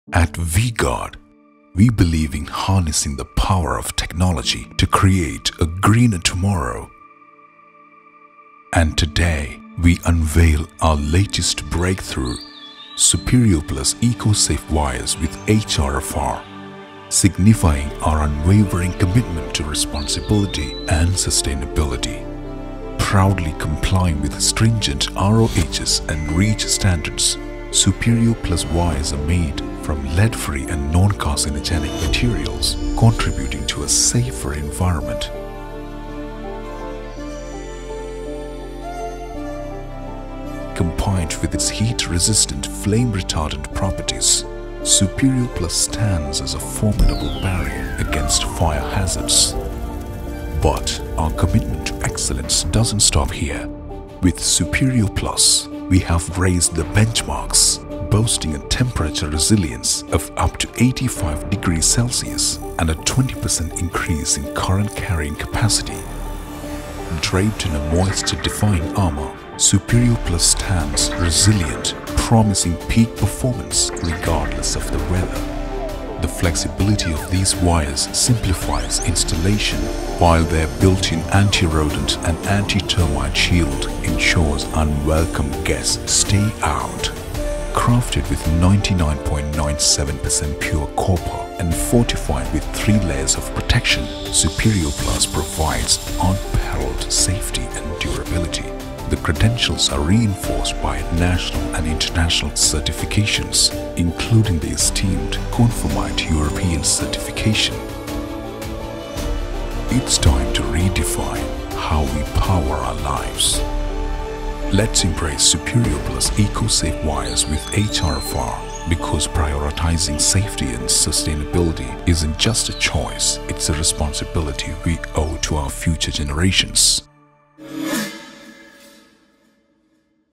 V-Guard Superio+, Long Lasting Eco-safe Wires
In our quest to create a greener, safer planet, we're proud to launch our Long Lasting Eco-safe Wires with Heat Resistant Flame Retardant (HRFR) property—V-Guard Superio+.

Compliant with ROHS and REACH, these wires don't contain harmful and carcinogenic chemicals leading to a better tomorrow